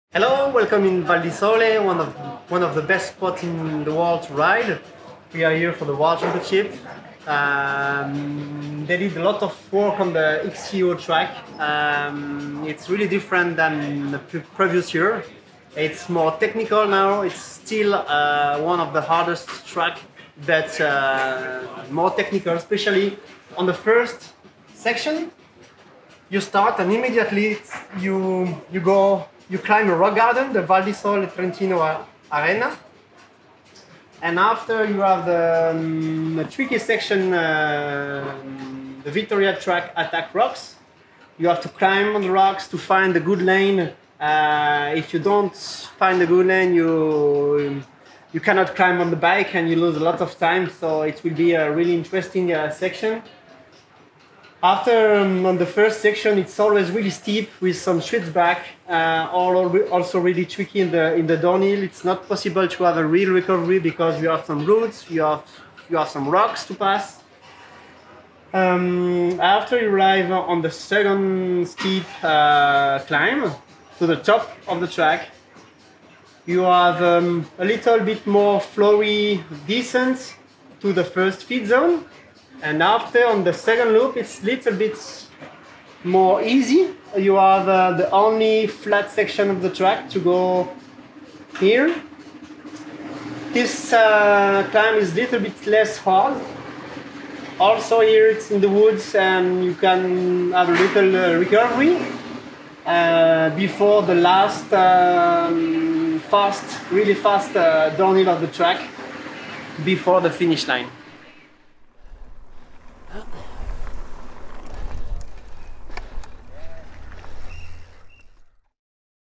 Vittoria XCO Course Preview with Julien Absalon | 2021 UCI MTB World Championships
Watch as two-time Olympic Champion, Julien Absalon presents the Vittoria XCO Course Preview at the 2021 UCI MTB World Championships in Val di Sole, Italy.